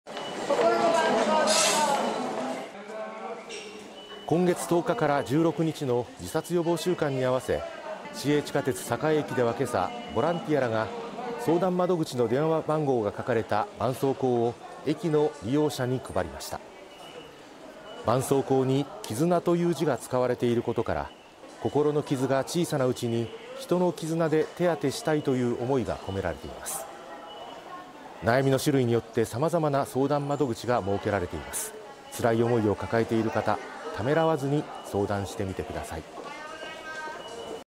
ためらわず窓口に相談を…9/10-16の自殺予防週間に合わせ“こころの絆創膏”配布 窓口の連絡先記載
9月10日から16日の#自殺予防週間 にあわせ、名古屋 では11日朝、悩みの相談窓口などが書かれた「こころの絆創膏」が配られました。

市営地下鉄の栄駅では11日朝、ボランティアらが相談窓口の電話番号が書かれた絆創膏を駅の利用者に配りました。

絆創膏に「絆」という字が使われていることから、「こころの傷が小さなうちに人の絆で手当てしたい」という思いが込められています。

悩みの種類によって様々な相談窓口が設けられています。つらい思いを抱えている方は、ためらわずに相談してみてください。

■名古屋市こころの健康無料相談
（052）962-9002
（午前9時～午後10時 祝日・年末年始除く）

2023年9月11日放送